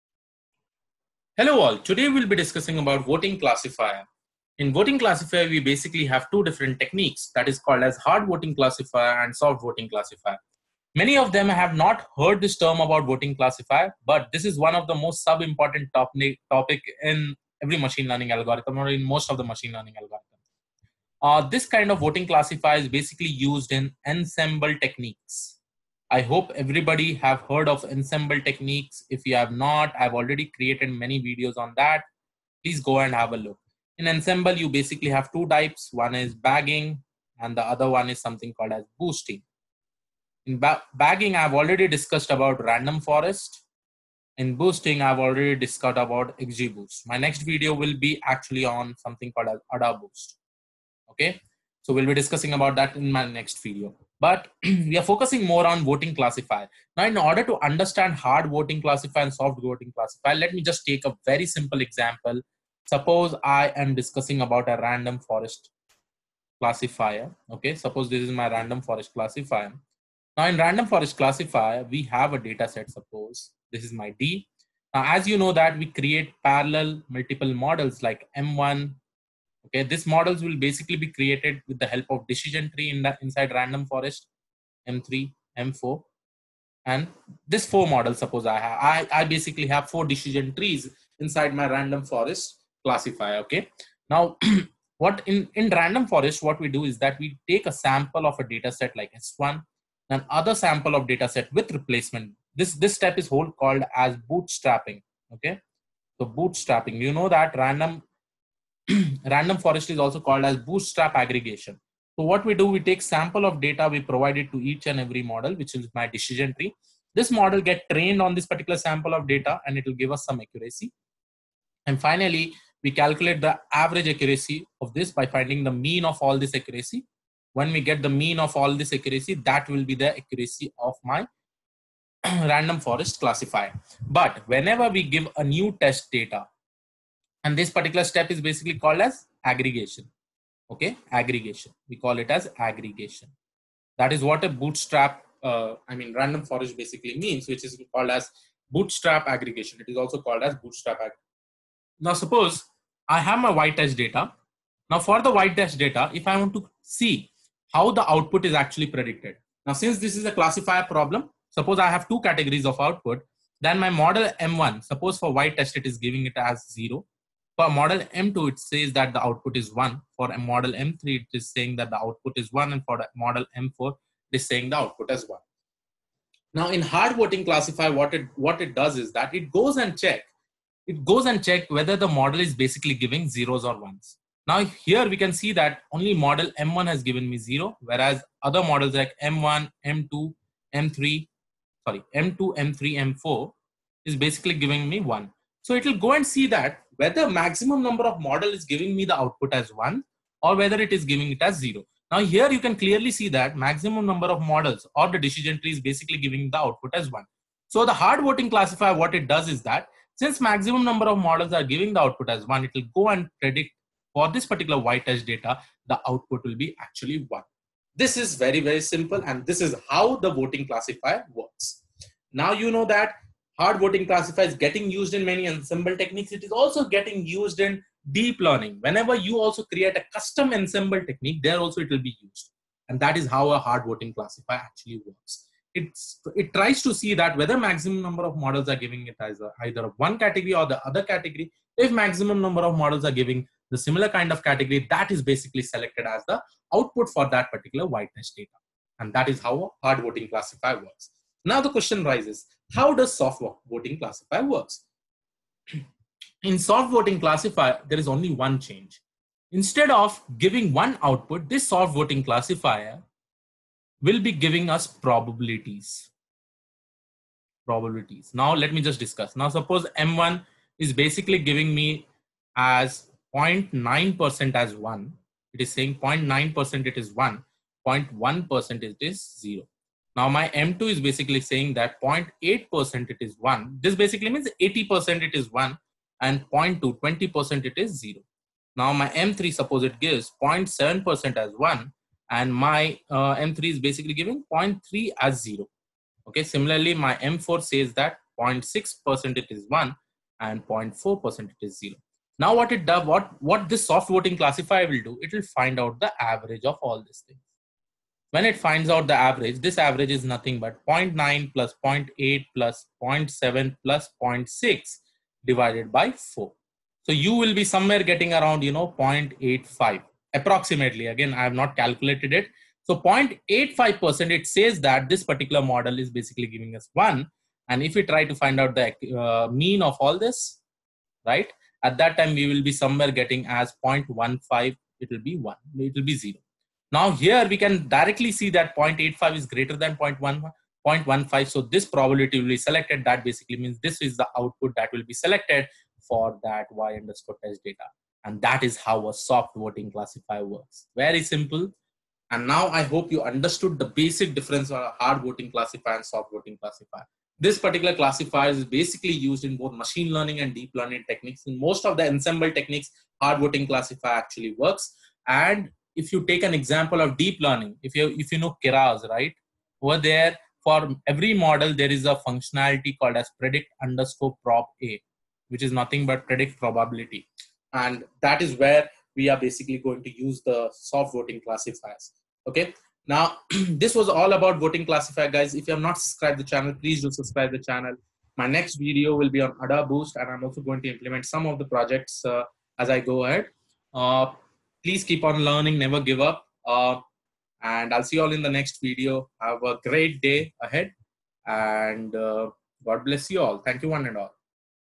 Voting Classifier(Hard Voting and Soft Voting Classifier)
The EnsembleVoteClassifier is a meta-classifier for combining similar or conceptually different machine learning classifiers for classification via majority or plurality voting.In hard voting, we predict the final class label as the class label that has been predicted most frequently by the classification models

You can buy my book on Finance with Machine Learning and Deep Learning from the below url

amazon url